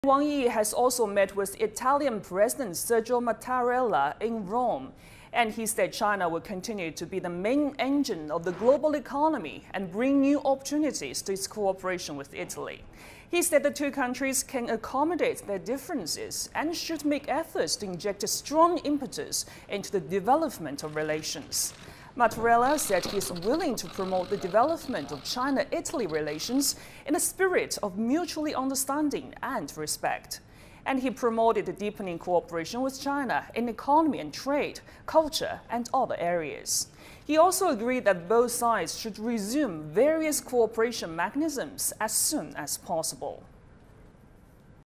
Wang Yi: China to bring new opportunities to cooperation with Italy
China will continue to be the main engine of the world economy and bring new opportunities to its cooperation with Italy, a senior Chinese diplomat said on Friday. Wang Yi, member of the Political Bureau of the Communist Party of China (CPC) Central Committee and director of the Office of the Foreign Affairs Commission, made the remarks when meeting with Italian President Sergio Mattarella in Rome, Italy.

Wang said that China and Italy can accommodate their differences from the broad perspective of history and civilization, respect each other's development path, and promote the sound and steady development of bilateral relations.

For his part, Mattarella said he is willing to promote the development of Italy-China relations in the spirit of mutual understanding and respect, deepen cooperation in economy and trade, culture and other areas.